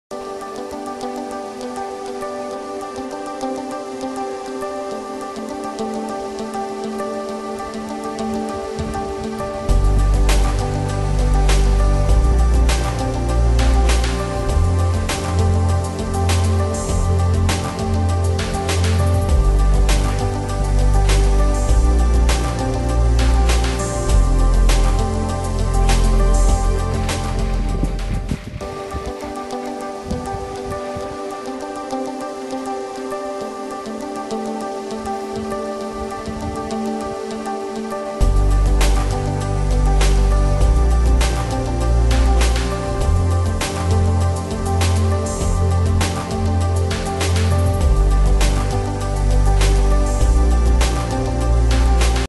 D'cent Roof - An automated opening & closing louvered roof
The automated Louvers within this Patio Roof are able to be turned up to 180 degrees for optimum shading. When flat the patio roof becomes watertight, protecting you from the Indian Weather conditions.
No, worry …..when you are out of your residence and rain comes sudden, now louvers will get automatically closed when the very first drop of rain comes at louvers.